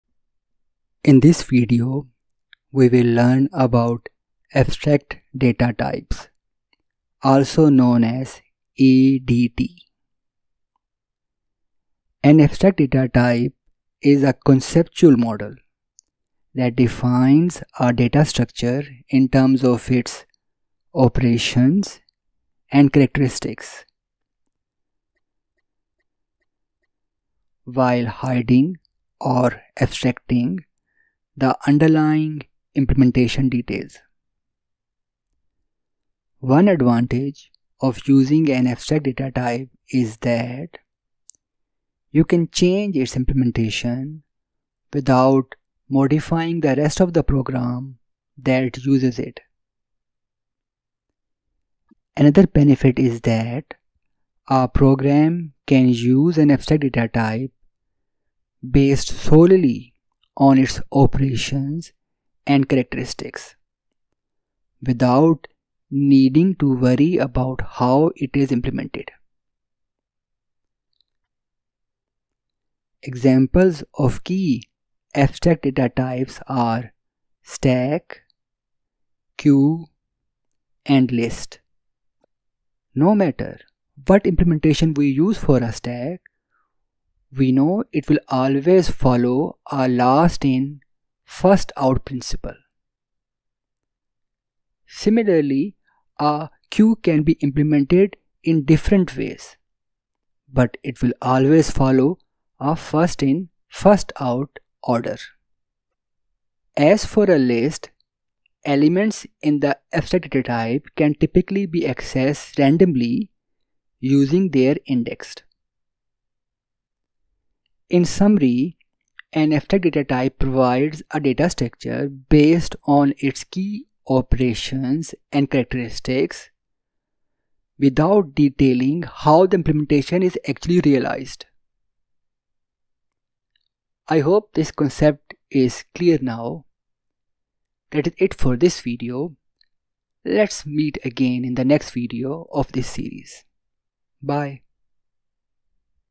Data Structures: What is an Abstract Data Type (ADT)?
This video defines an Abstract Data Type (ADT), provides its key advantages, and give examples of key abstract data types.

I would greatly appreciate it if you could treat me to a cup of coffee or show support for my channel through any other means. You can do so by visiting: